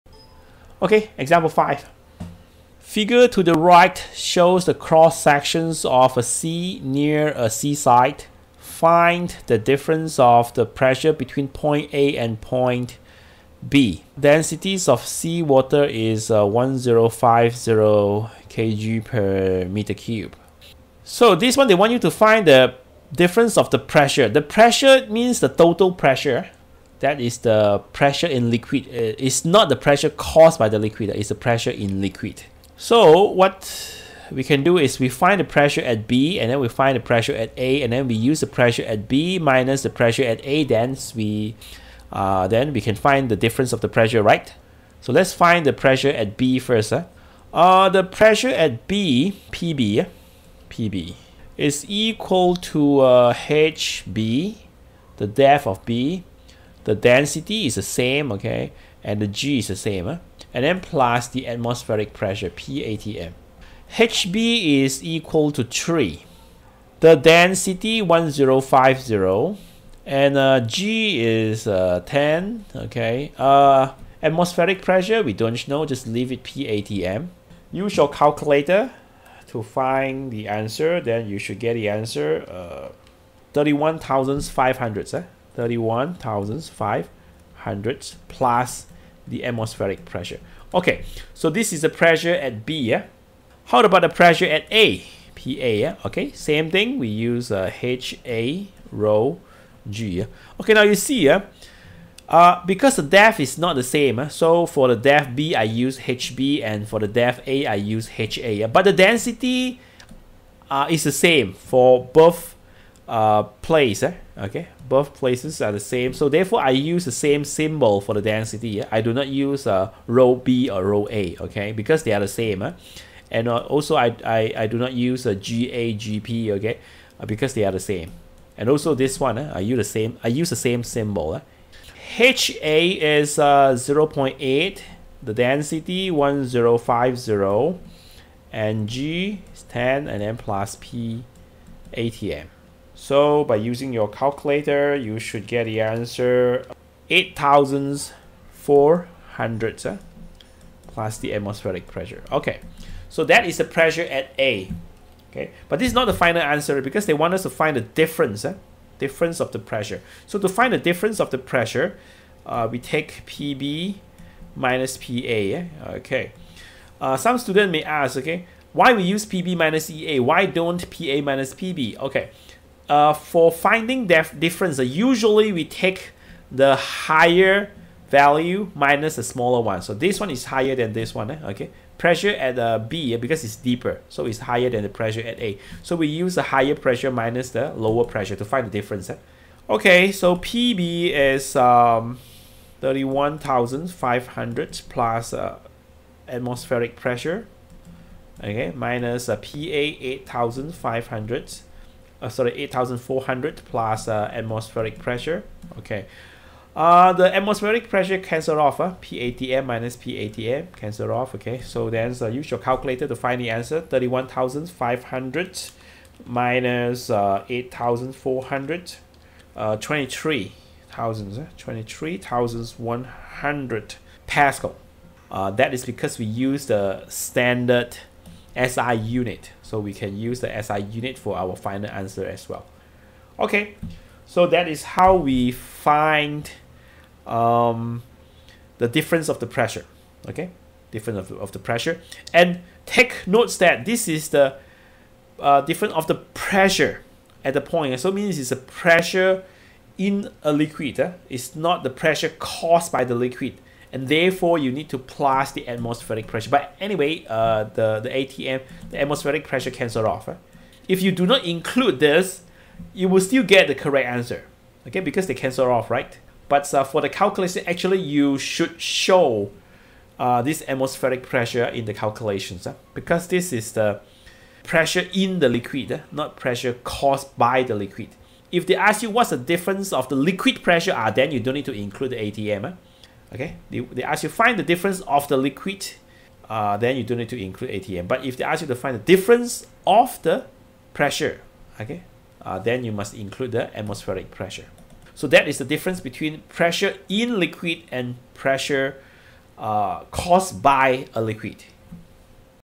Solving Problems Involving Pressure in Liquids
Form 5 Physics KSSM Chapter 2 - Pressure

1. Pressure in liquid is owing to the weight of the liquid acting on the surface of any objects in the liquid.
2. Pressure of a liquid is directly proportional to
a. the gravitational field strength
b. the depth
c. density of the liquid.